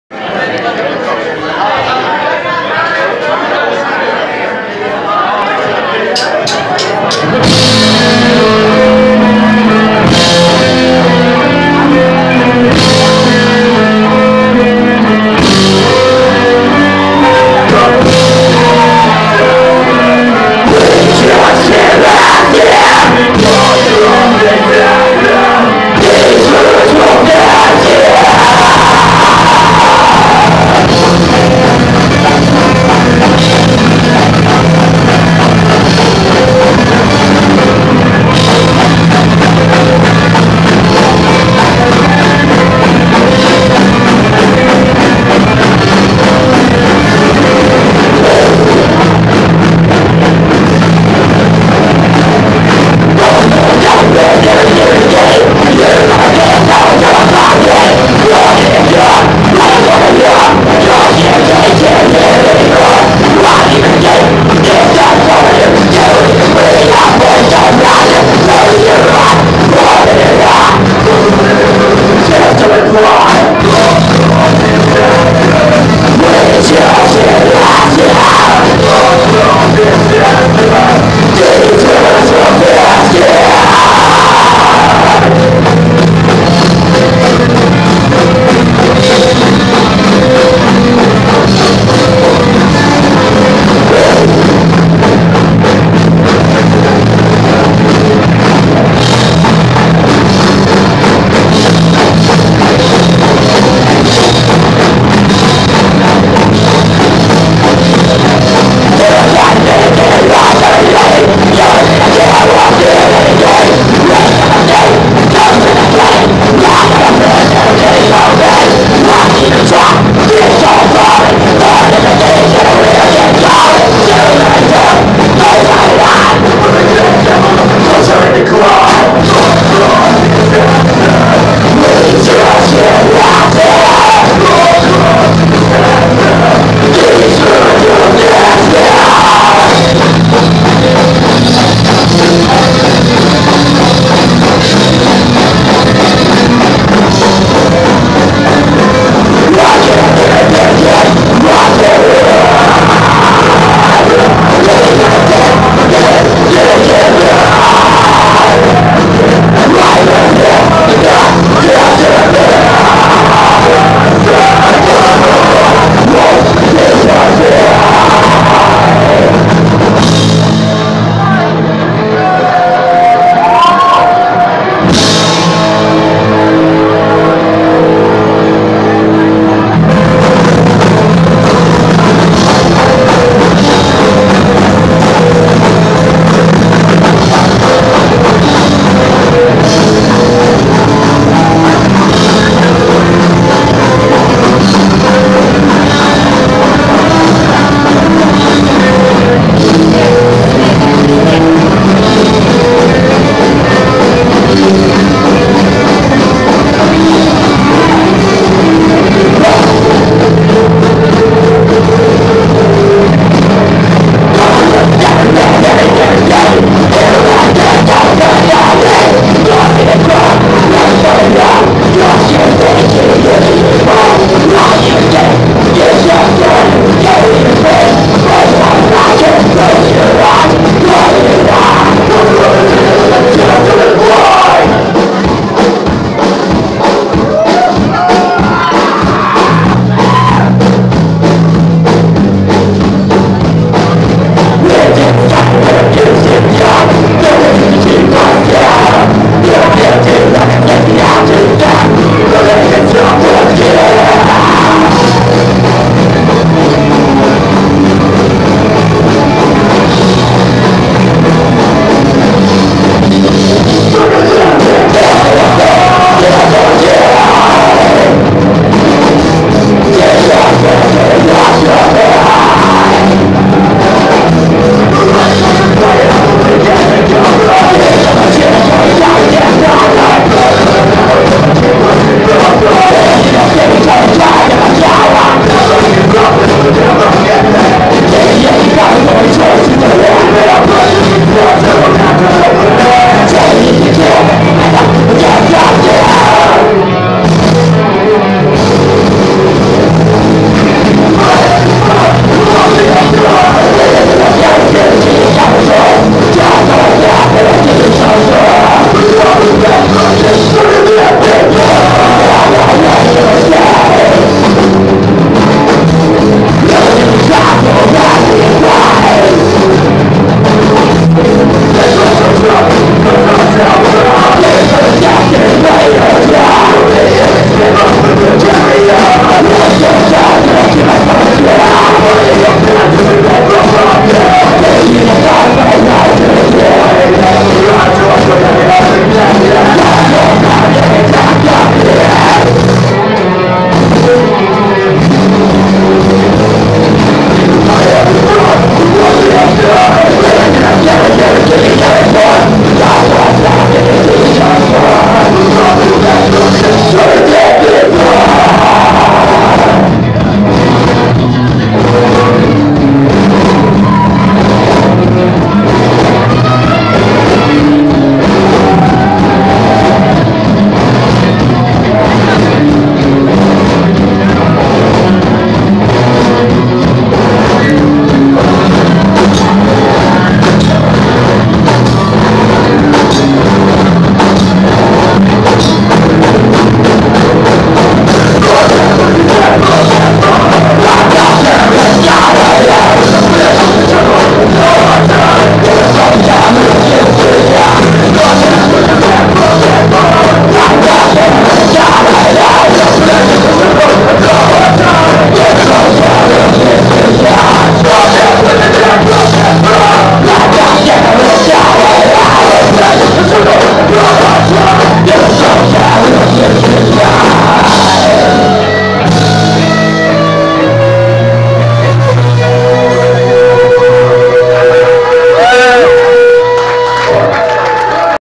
10-6-18 Violent Affair - Live at the Blue Note
10-6-18. Violent Affair performing live at The Blue Note in OKC.